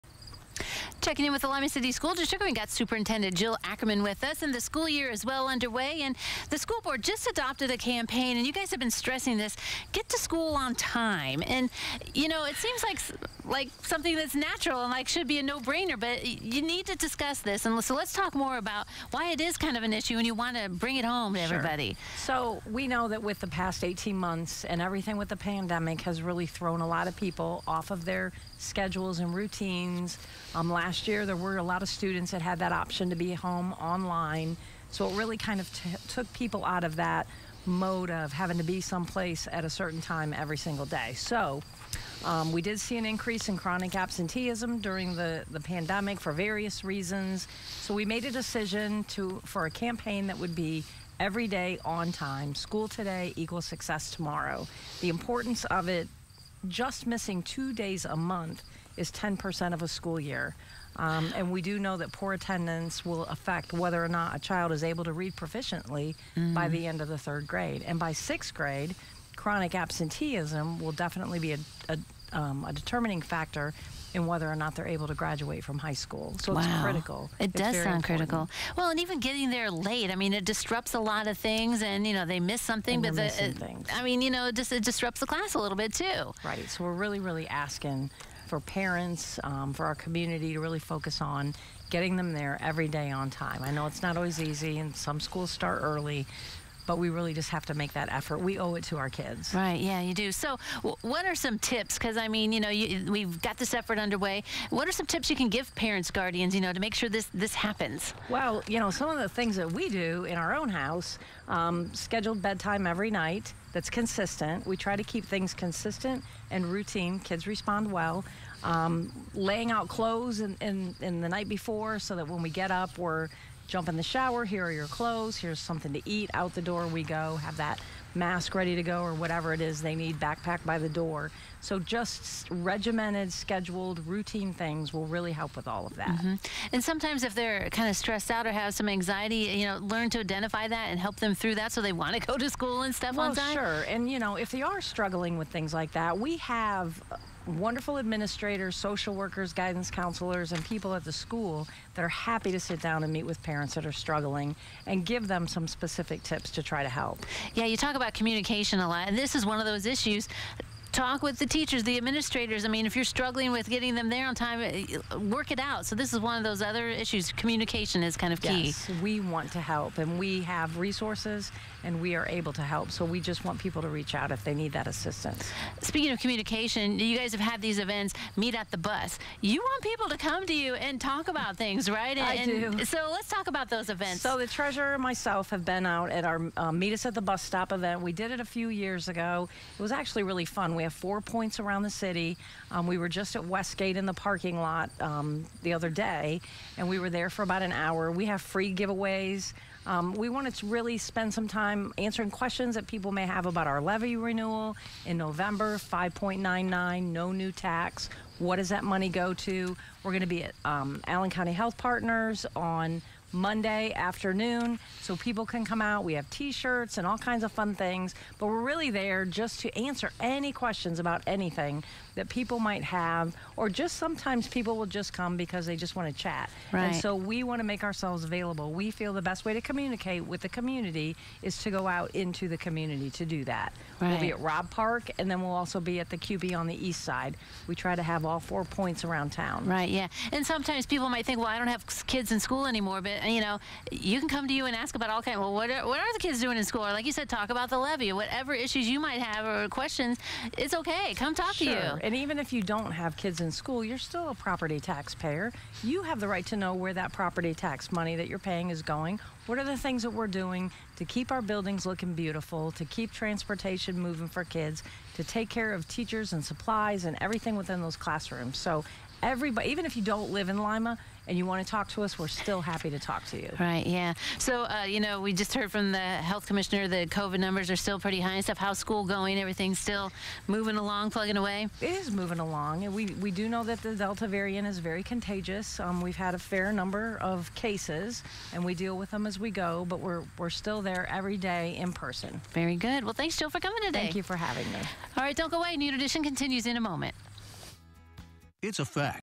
Lima City School District
Superintendent Jill Ackerman talks more about the Every Day on Time Campaign. Plus the district is out in the community at the Meet Us at the Bus Stop events.